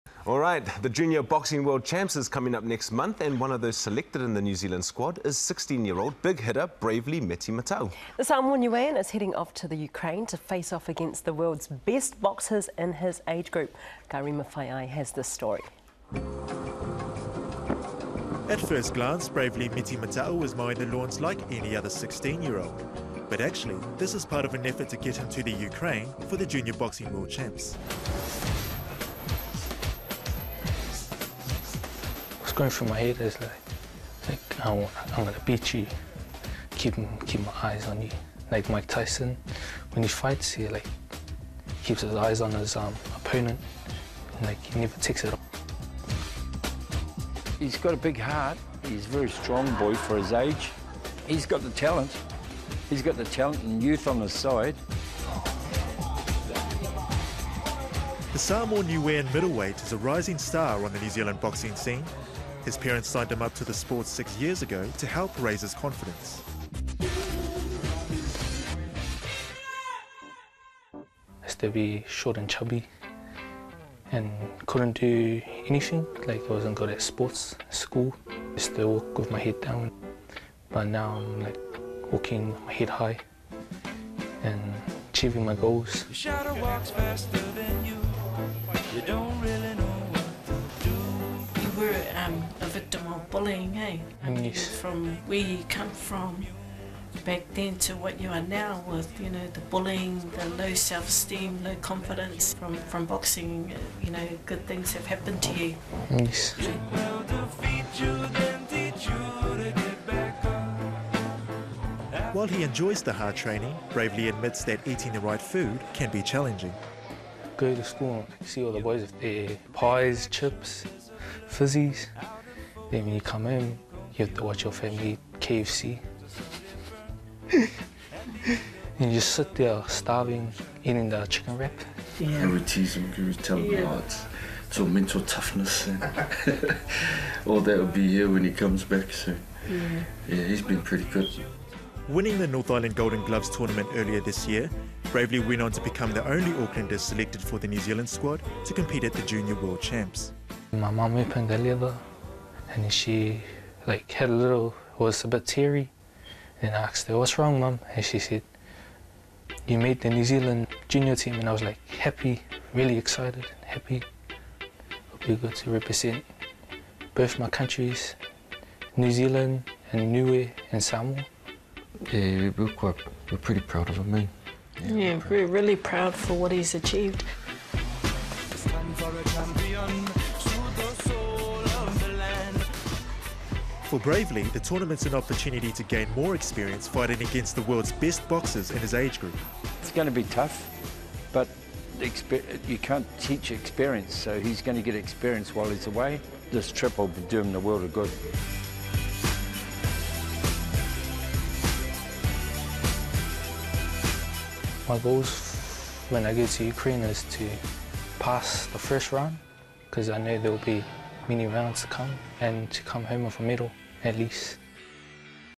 Samoan Niuean heading to Junior Boxing World Champs in Ukraine
16 year old Bravely Meti Mata'u is the only Aucklander to be selected for the NZ junior boxing squad this year and is heading to the Ukraine for the AIBA Junior World Championships.